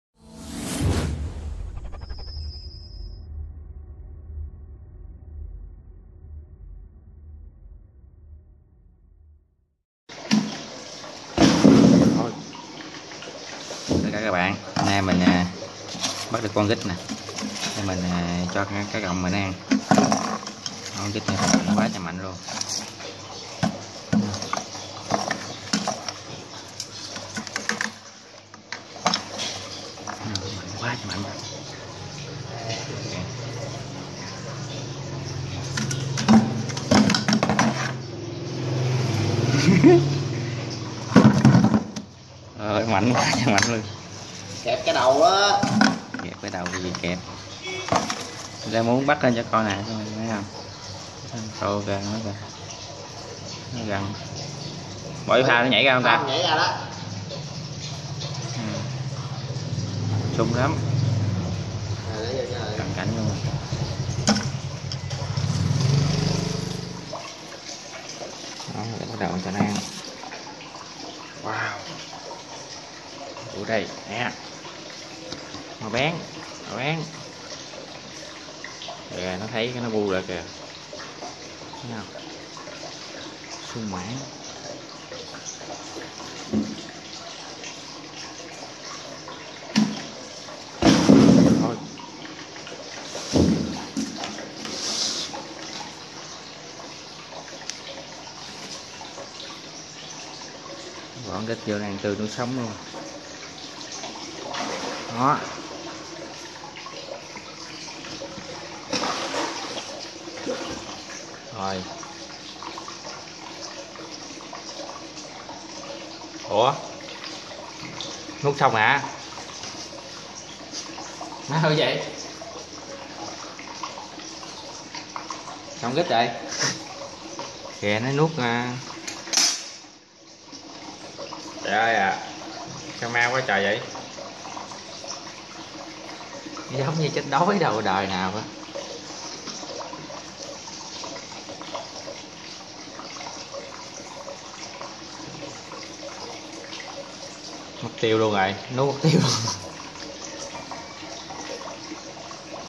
Cá Rồng Ngân Long Ăn Rết Trong 10 Giây Thật Không Thể Tin Được| Cá Rồng Ăn Rết Arowana Eat Centipede
Cá Rồng Ngân Long Ăn Rết Trong 10 Giây Thật Không Thể Tin Được | Cá Rồng Ăn Rết - Arowana Eat Centipede | MIỀN TÂY FOOD